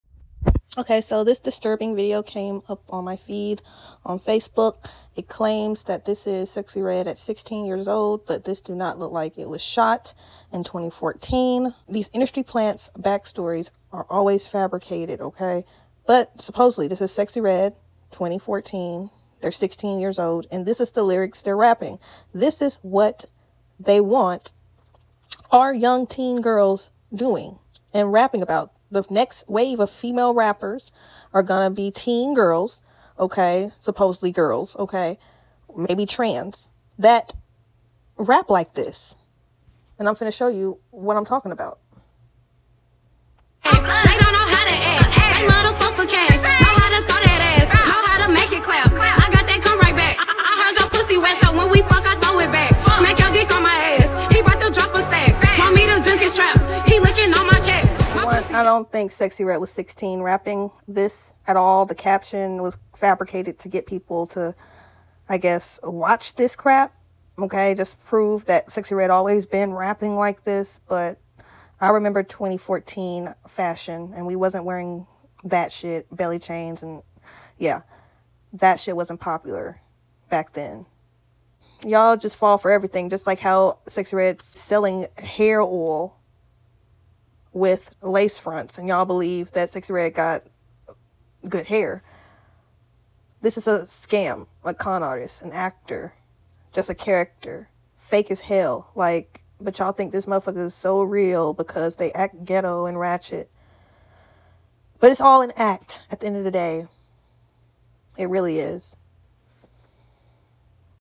Sexyy Reds Agenda is VERY obvious
They planted this character to DESTROY THE BLACK YOUTH!